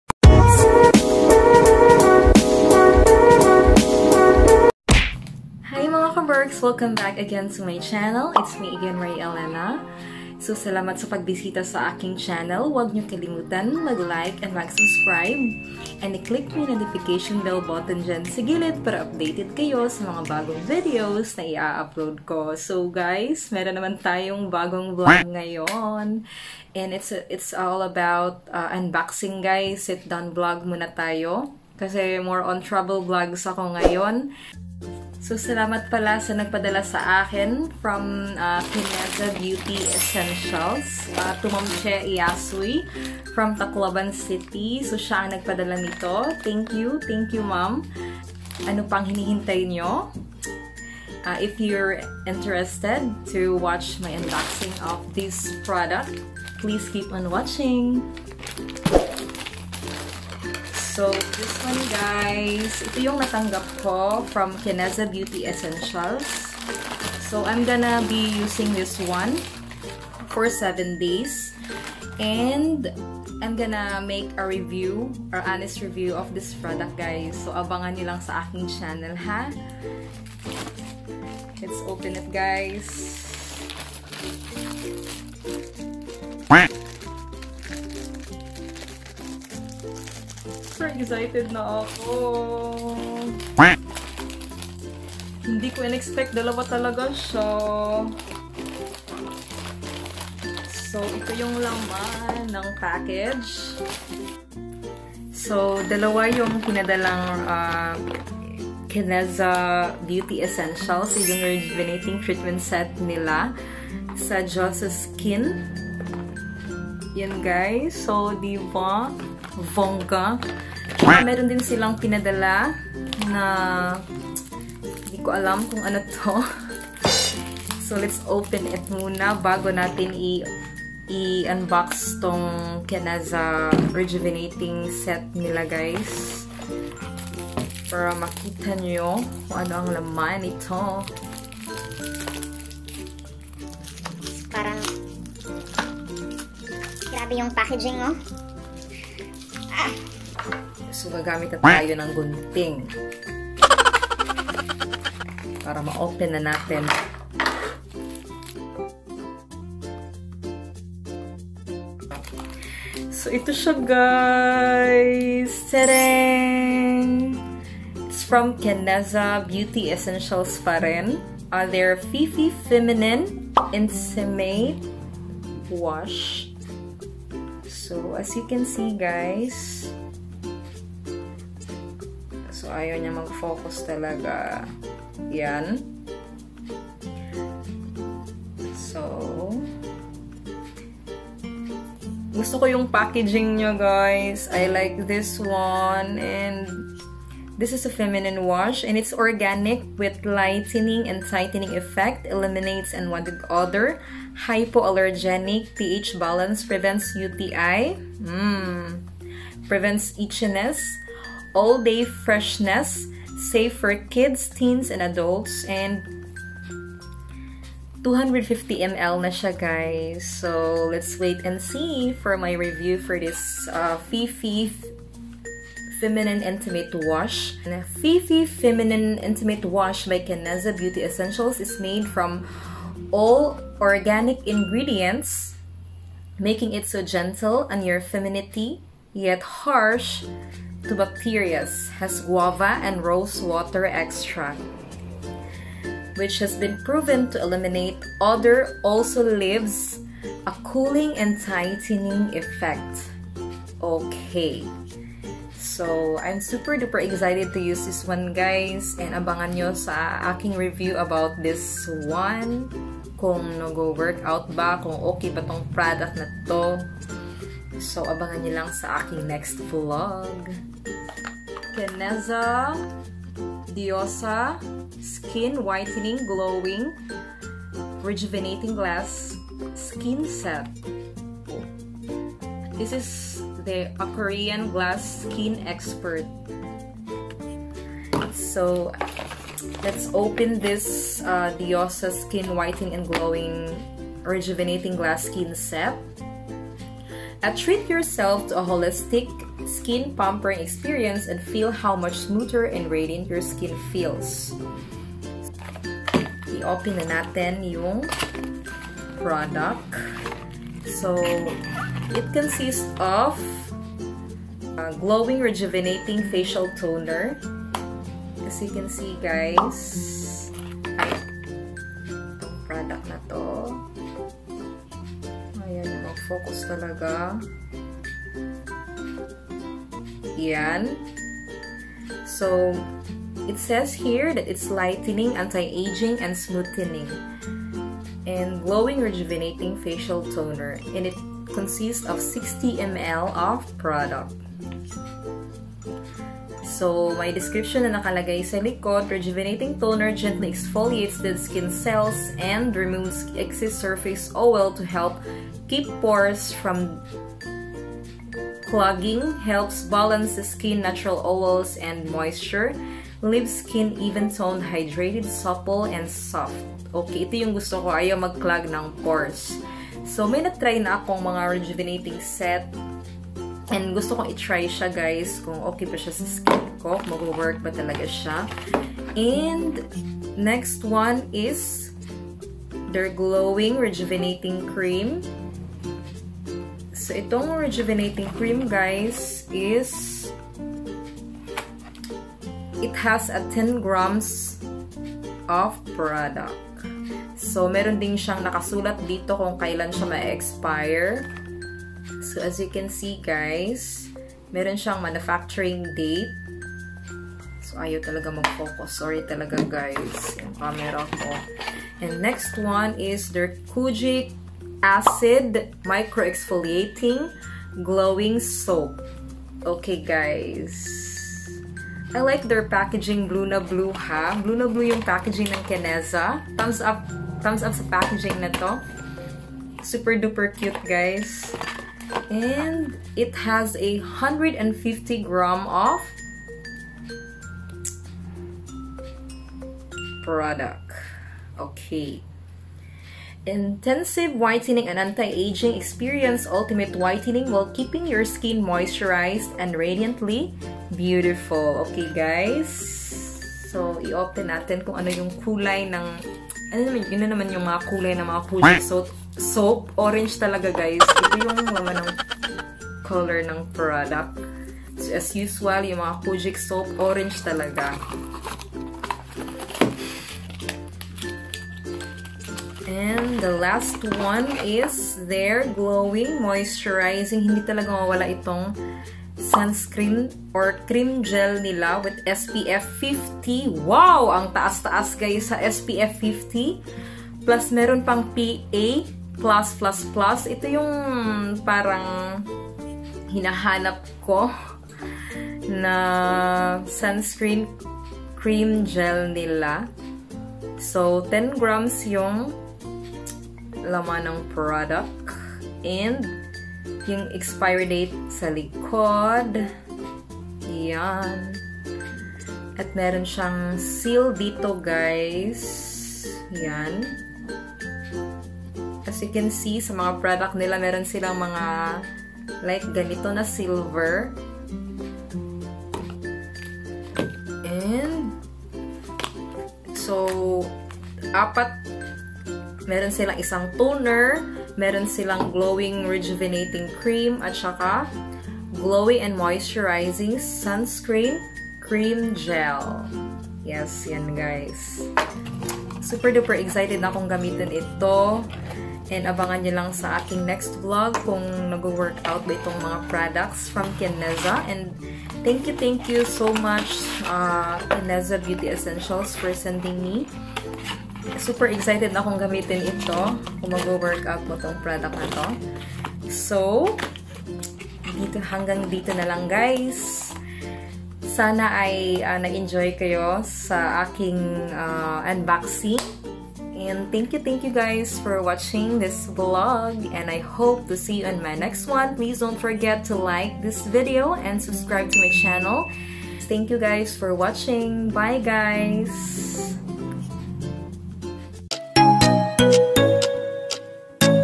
PR UNBOXING KINEZA BEAUTY ESSENTIALS
hi guys! im Ma Elena Canencia from Ormoc City, Leyte

Thank you Kineza Beauty Essentials for sending me this PR products. It's a blessing for me this year. Soon on my YouTube Channel abangan nyo ang aking honest review sa about this products.❤️